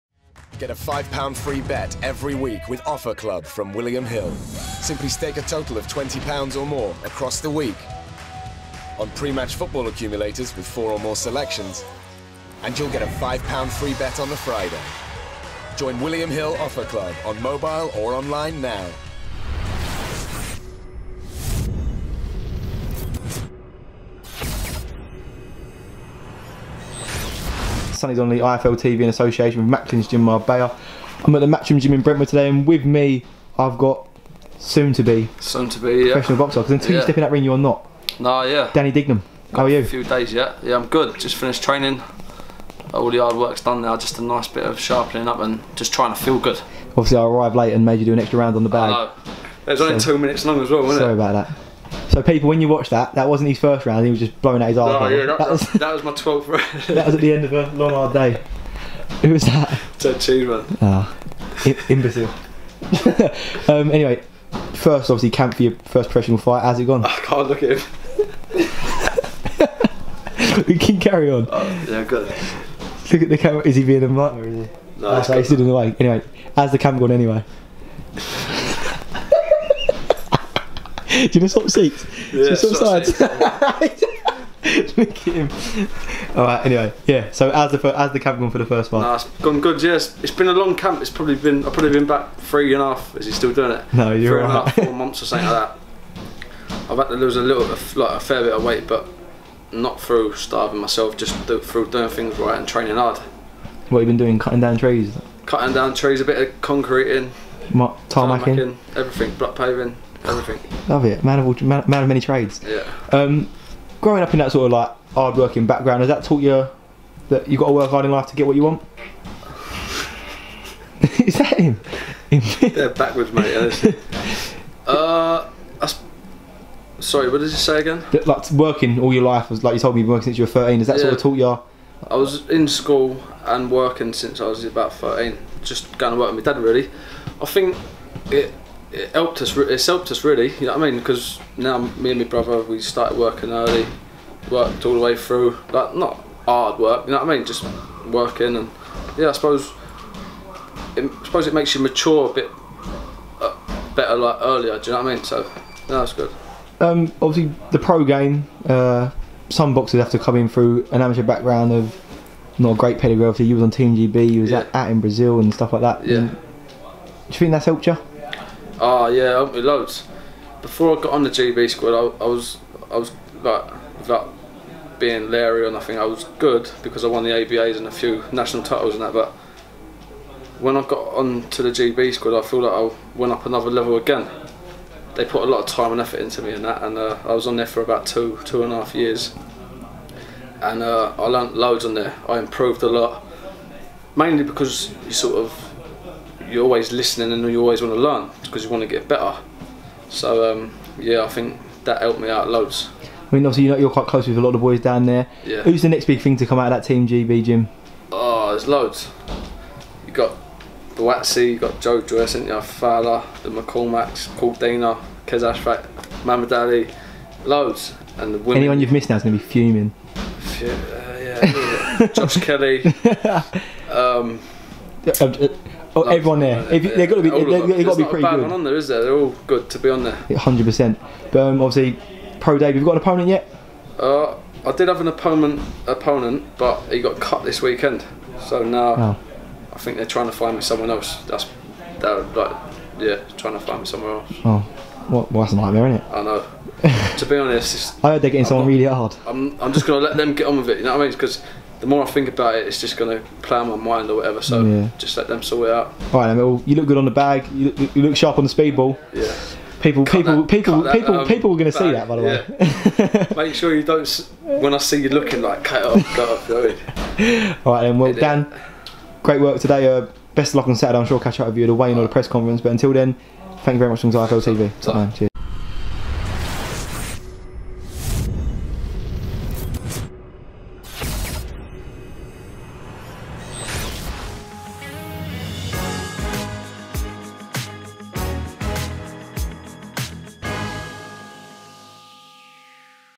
'ME & MY BROTHER WORKED SINCE WE WERE 13, THAT HELPED US' - DANNY DIGNUM SET FOR PRO-DEBUT ON NOV 26
'ME AND MY BROTHER WORKED SINCE WE WERE 13, THAT HELPED US' - DANNY DIGNUM SET FOR PRO-DEBUT ON NOV 26

INTERVIEW BY SONNY DONNELLY